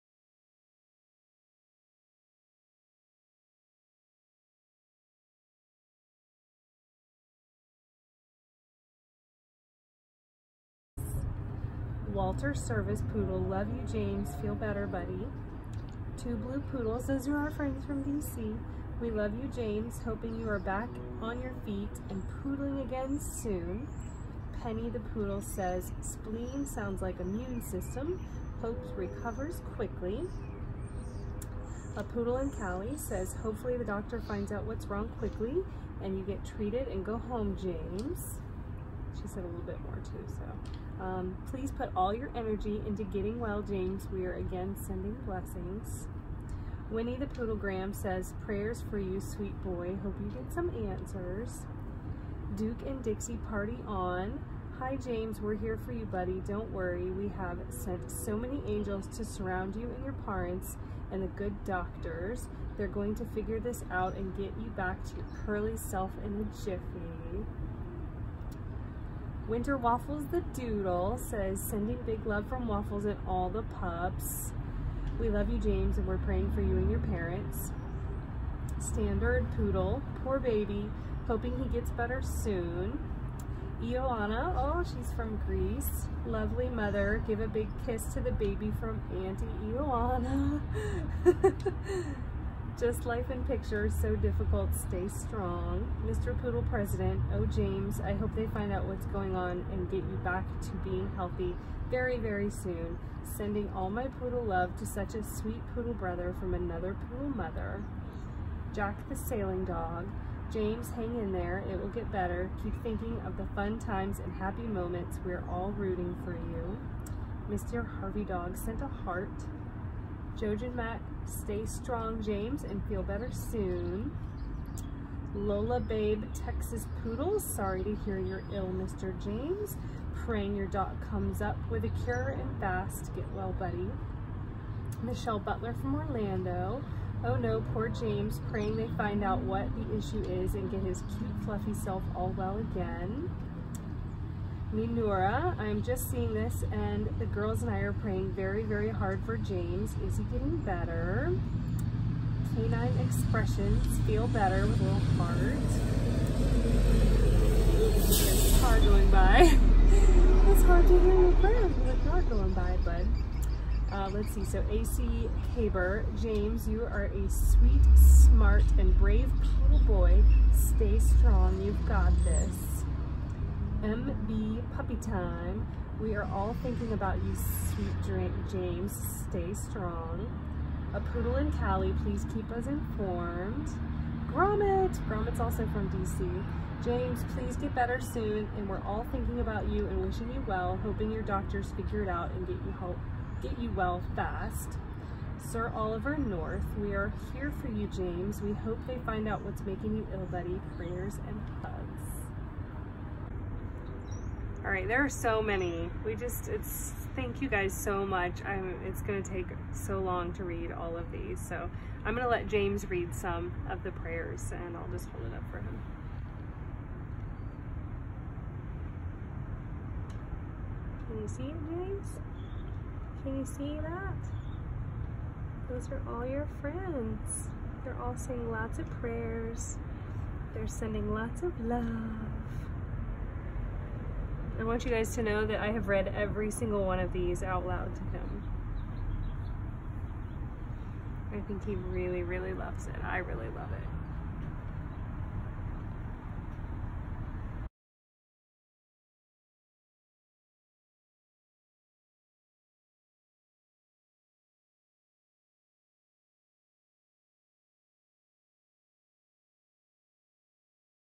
Prayers for James
These are some of the many prayers James received from his Instagram friends when he was sick this Spring 2019.  We are forever thankful for these prayers.  Every day we visited James in the ICU, I read these prayers out loud to him.  Usually with tears in my eyes and tears running down my cheeks.

When we were in Europe, James only drank bottled water.  When we came back to the US this year, we gave James tap water.  James quickly got tear stains on his face - those reddish rust colored areas around his eyes and mouth.  We talked to a vet that said he probably wasn't used to the pH of the tap water in Florida and we should switch him back to bottled water.  James is now back on bottled distilled water and all of those tear stains you see in this video are gone.

We don't know what caused James to get sick.  He had an anal gland that was infected.  Then the vets think he had a reaction to the antibiotics.  James had two 4 day visits to the ICU where he stayed on fluids.  We are so thankful he is now recovering!

Check out our blog to read more details of James being in the ICU:
OurBigAdventure.com

Follow James on Instagram:
JamesThePoodle